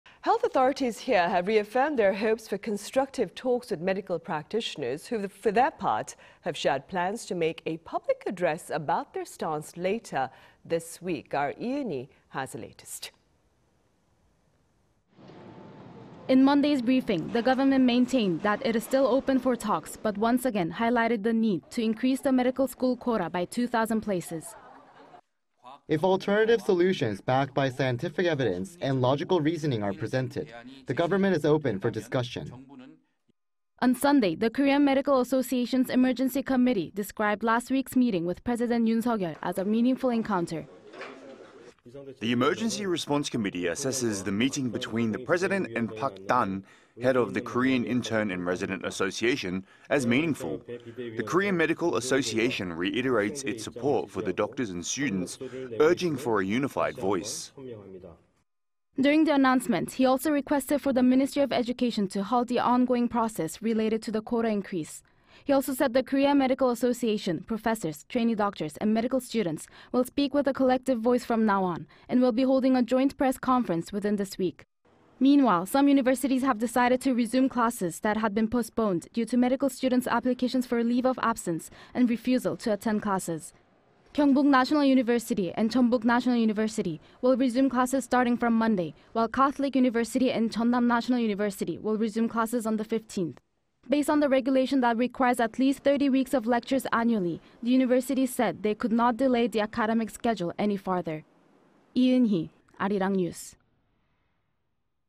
Korean Medical Association calls meeting with President "meaningful" while medical dispute continues
의협 "대통령 면담 의미 있었다" 합동 기자회견 예고

Health authorities here have reaffirmed their hopes for constructive talks with medical practitioners who for their part have shared plans to make a public address about their stance later this week.
Lee Eun-hee  has the latest.

In Monday's briefing, the government maintained that it is still open for talks, but once again highlighted the need to increase the medical school quota by 2-thousand places.

"If alternative solutions backed by scientific evidence and logical reasoning are presented, the government is open for discussion."

On Sunday, the Korean Medical Association's emergency committee described last week's meeting with President Yoon Suk Yeol as a "meaningful encounter."

"The Emergency Response Committee assesses the meeting between the President and Park Dan, head of the Korean Intern and Resident Association, as meaningful. The Korean Medical Association reiterates its support for the doctors and students, urging for a unified voice."

During the announcement, he also requested for the Ministry of Education to halt the ongoing process related to the quota increase.
He also said the Korea Medical Association, professors, trainee doctors and medical students will speak with a collective voice from now on, and will be holding a joint press conference within this week.

Meanwhile, some universities have decided to resume classes that had been postponed due to medical students' applications for leave of absence and refusal to attend classes.
Kyungpook National University and Jeonbuk National University will resume classes starting from Monday, while Catholic University and Chonnam National University will resume classes on the 15th.
Based on the regulation that requires at least thirty weeks of lectures annually, the universities said they could not delay the academic schedule any further.

Lee Eun-hee, Arirang News.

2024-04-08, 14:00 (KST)